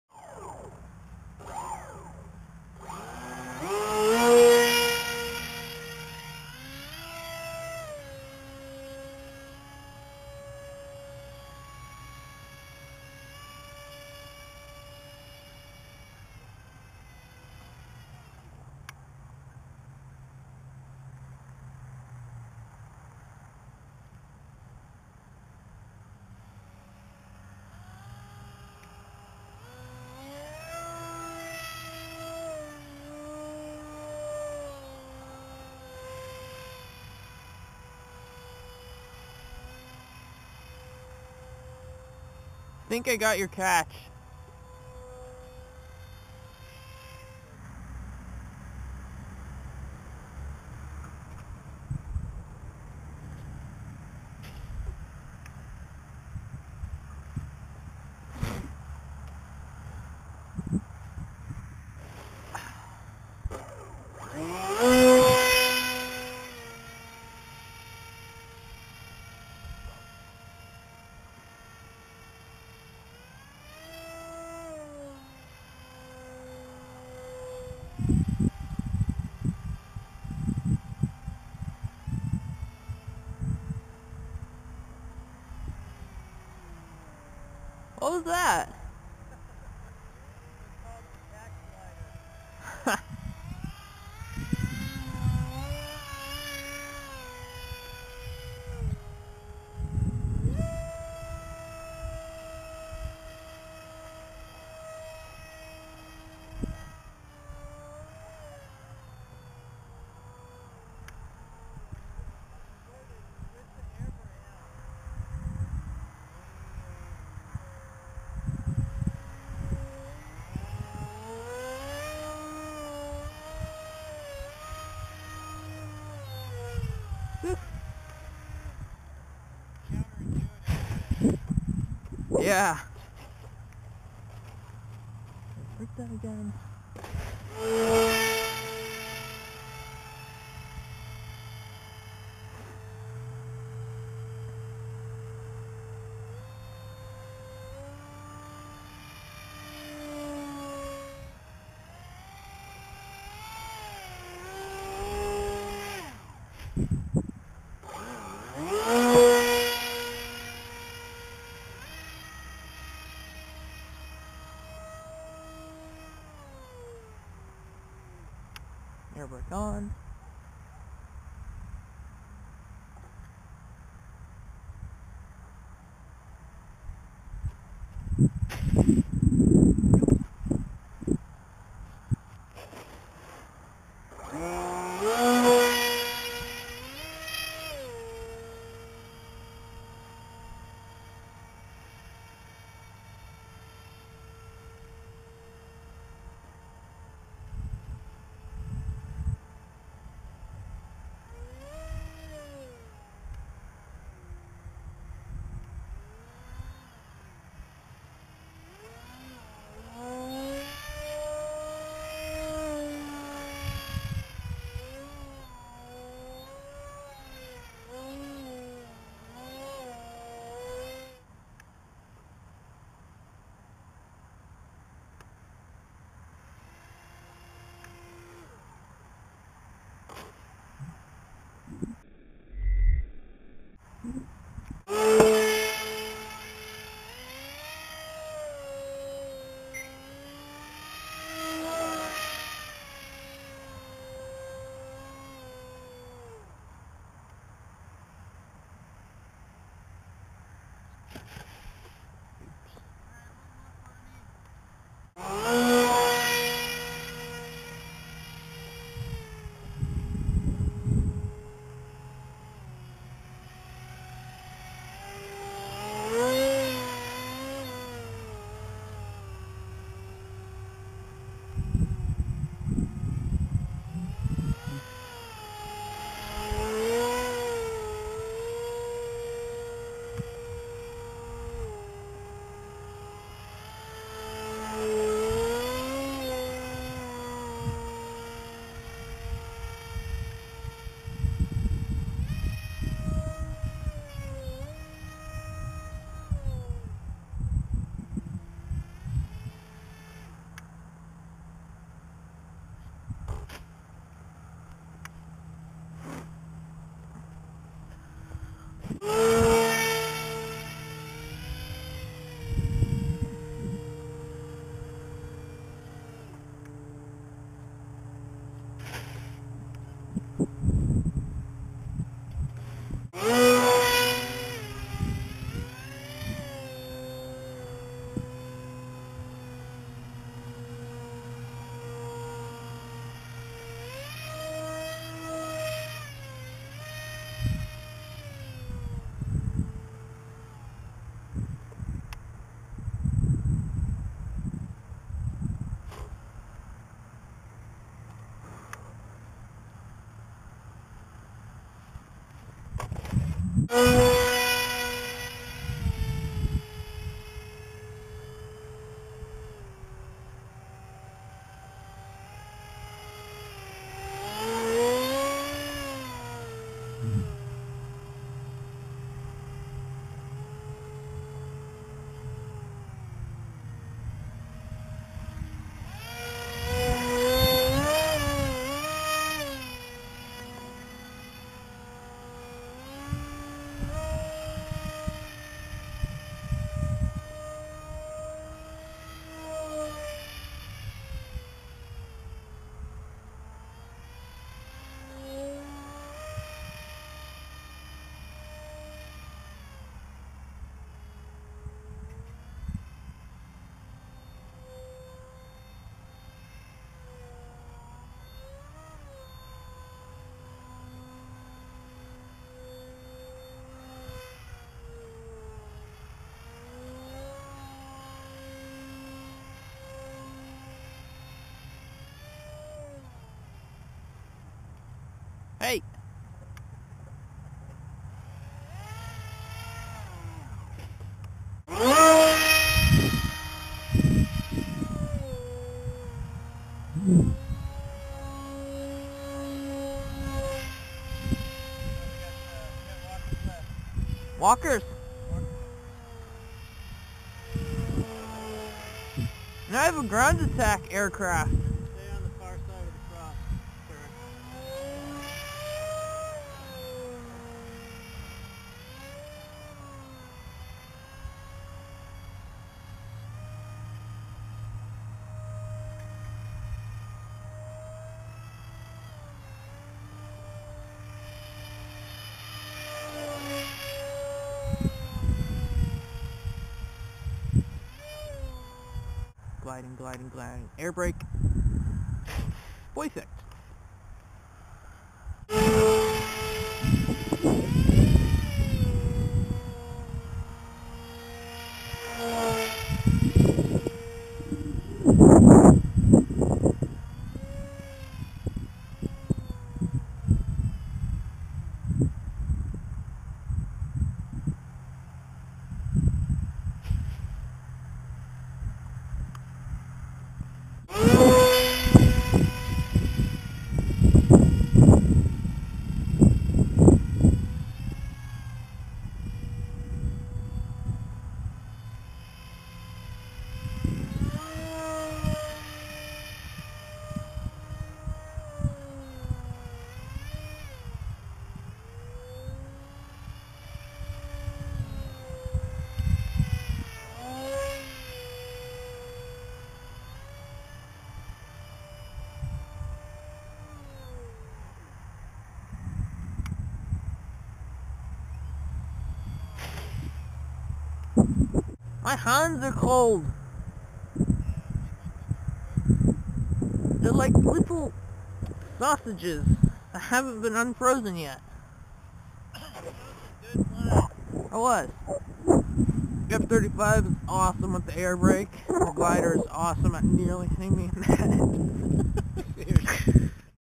RCPowers F-35v2 airbrake Demonstration
this is my f-35v2 from RCPower.com that i modified to have an air break to slow it down for landing in small spaces.